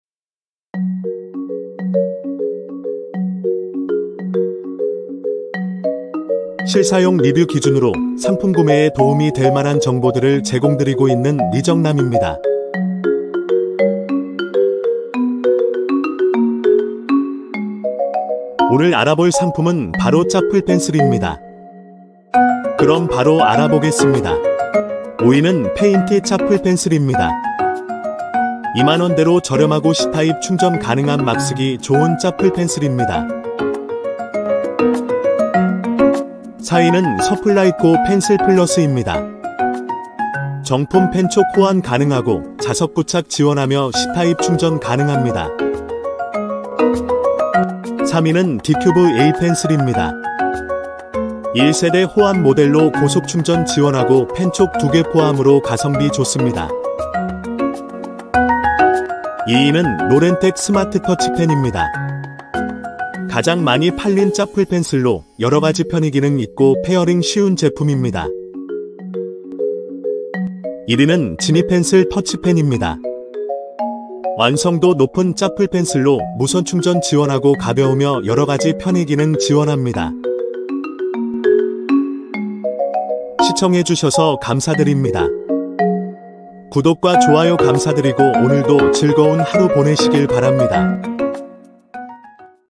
짭플펜슬 추천 BEST 5! (실사용 리뷰 기준으로 정리)
0:25
5위
페인티 짭플펜슬 (2만원)

0:37
4위
서플라이코 펜슬 플러스 (2만 7천원)

0:48
3위
디큐브 에이펜슬 (2만 2천원)

0:59
2위
로랜텍 스마트터치펜 (2만 9천원)

1:11
1위
지니펜슬 터치펜 (3만 8천원)